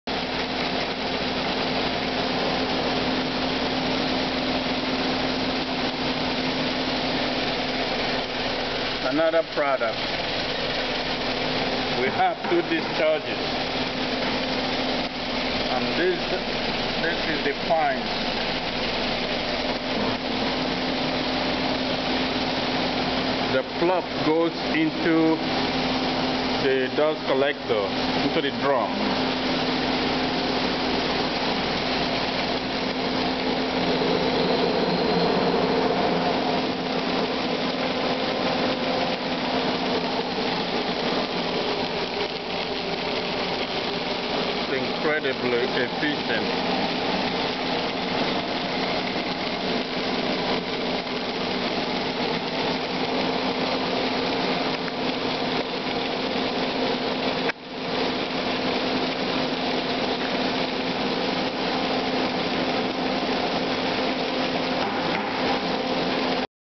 Kason Test #1 for ADS
Separation of fluff, paper, strings, etc from post consumer shredded plastic